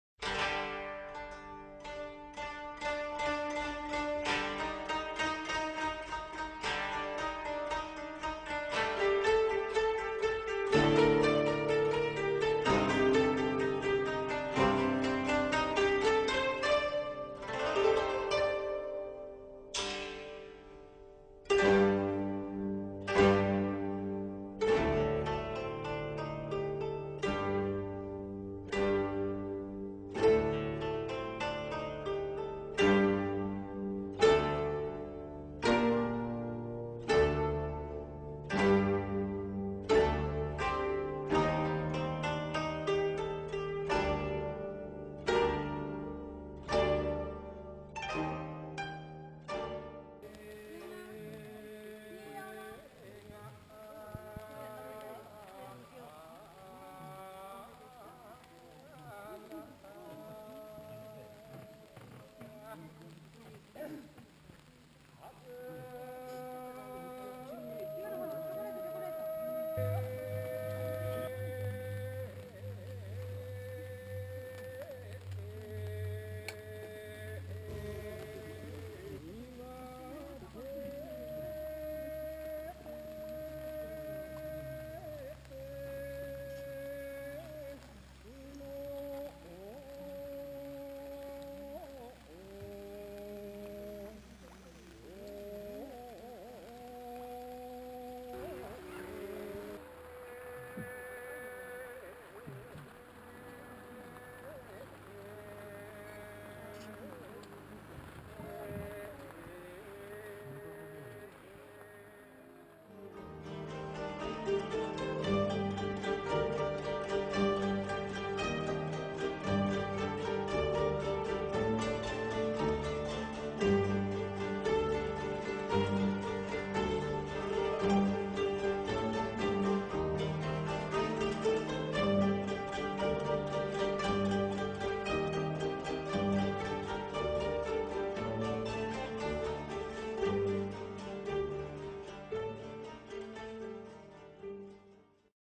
Geibikei Gorge
Geibikei Gorge Japan. 猊鼻渓日本！A little video of Geibikei Gorge! Gorgeous Canyon! And some video of the boat captain singing a japanese folk song. Sorry my mic is really bad so you have to really turn it up to hear the japanese folk song!